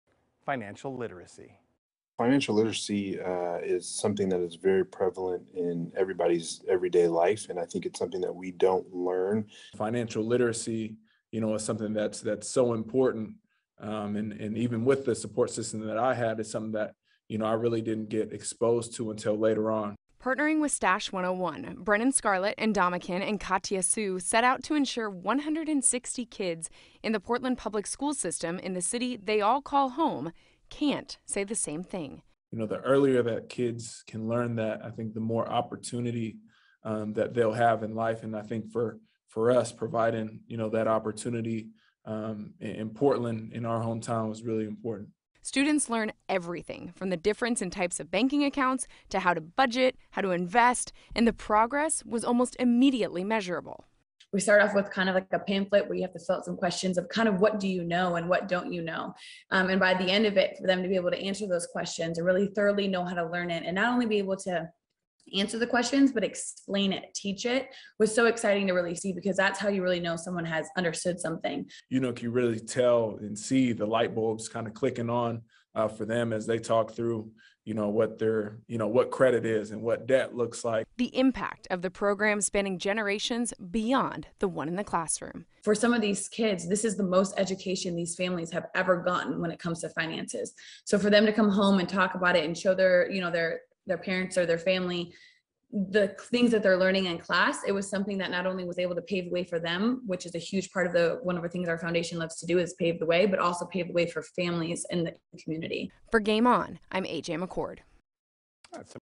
Stash 101 | Local NFL players team up to tackle financial literacy
Financial literacy is something used every day, but rarely taught in schools. That's a gap NFL players and Portland natives, Ndamukong Suh and Brennan Scarlett, along with Ndamukong's wife, Katya, are working to fill. Partnering with Stash 101 this summer they helped more than 100 Portland kids understand the basics of financial literacy, to make a difference in the community they all call home.